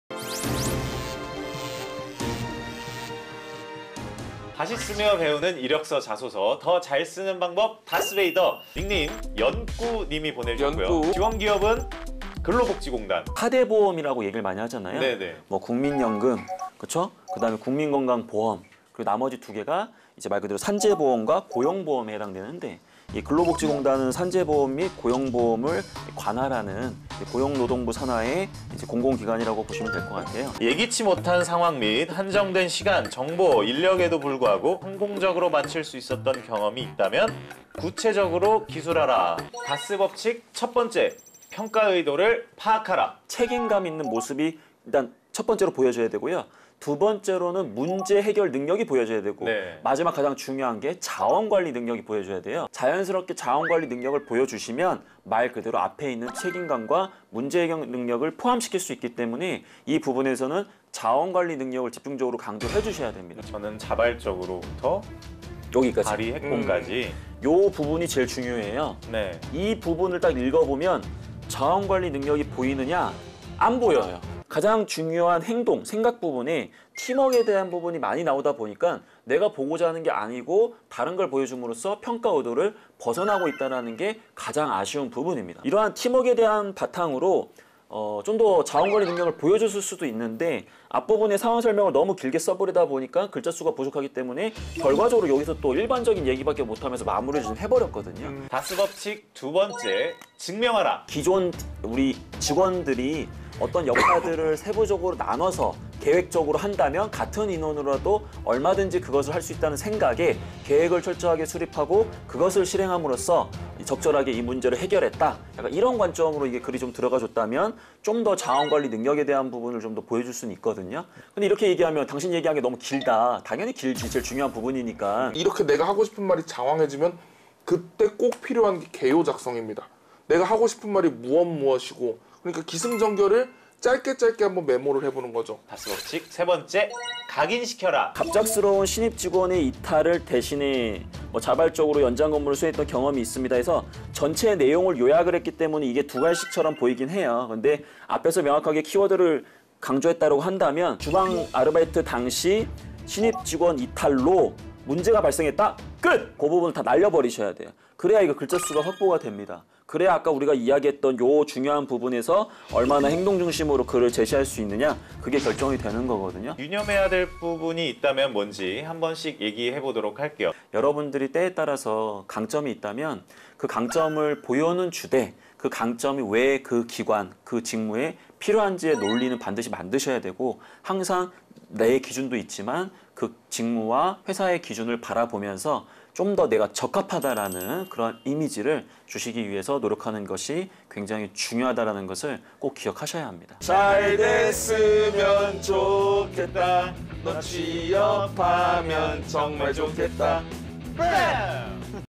[다쓰배이더] 질문 의도를 정확하게 파악하는게 첫번째! 근로복지단 자소서 첨삭 3분 만에 뿌셔뿌셔!
질문은 자원관리능력을 물었지만, 쓰고 나니 팀워크에 중점을 둔 자소서가 되었을 때..
평가 의도에서 벗어난 답변 쓸 때가 한두번이 아니지?
알면서도 틀릴 수 밖에 없는 질문 의도! 다쓰배이더 보고 3분 만에 뿌셔보자!
이 콘텐츠의 고화질 풀 영상을 보고 싶다면?
아래 링크를 클릭하세요!